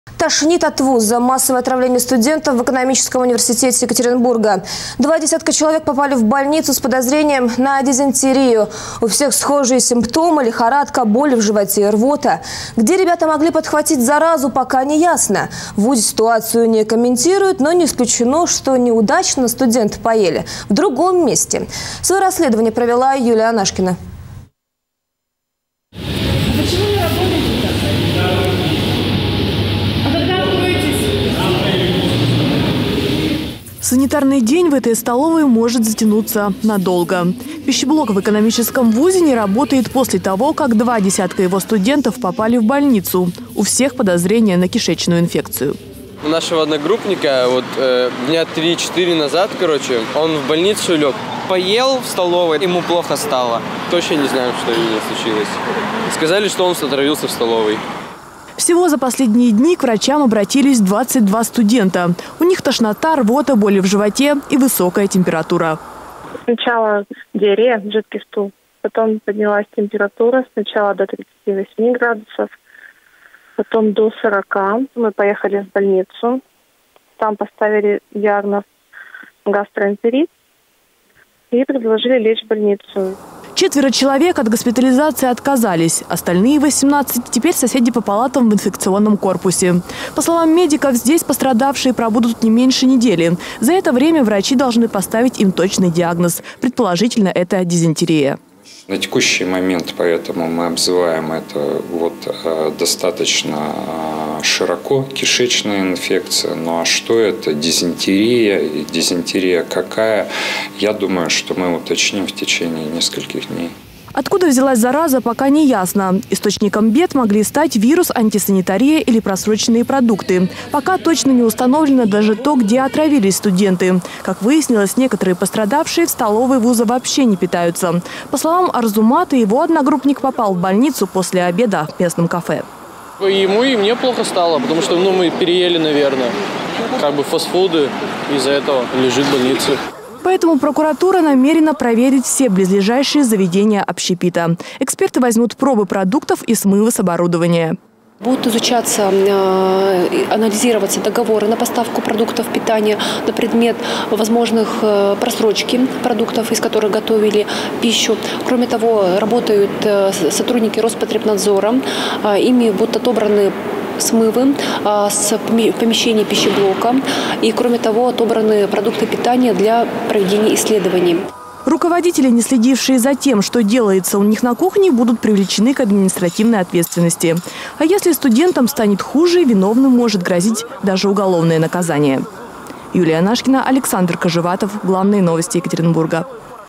В Екатеринбурге отравились студенты
Массовое отравление в вузе: два десятка студентов УрГЭУ подхватили острую кишечную инфекцию

В университете ситуацию комментировать отказываются

Санитарный день в этой столовой может затянуться надолго. Пищеблок в экономическом вузе не работает после того, как два десятка его студентов попали в больницу. У всех - подозрение на кишечную инфекцию. Всего за последние дни к врачам обратились 22 студента. У них тошнота, рвота, боли в животе и высокая температура. Четверо человек от госпитализации отказались, остальные 18 - теперь соседи по палатам в инфекционном корпусе. По словам медиков, здесь пострадавшие пробудут не меньше недели. За это время врачи должны поставить им точный диагноз. Предположительно, это дизентерия. Откуда взялась зараза, пока не ясно. Источником бед могли стать вирус, антисанитария или просроченные продукты. Пока точно не установлено даже то, где отравились студенты. Как выяснилось, некоторые пострадавшие в столовой ВУЗа вообще не питаются. По словам Арзумата, его одногруппник попал в больницу после обеда в местном кафе. Поэтому прокуратура намерена проверить все близлежащие заведения общепита. Эксперты возьмут пробы продуктов и смывы с оборудования. Руководители, не следившие за тем, что делается у них на кухне, будут привлечены к административной ответственности. А если студентам станет хуже, виновным может грозить даже уголовное наказание.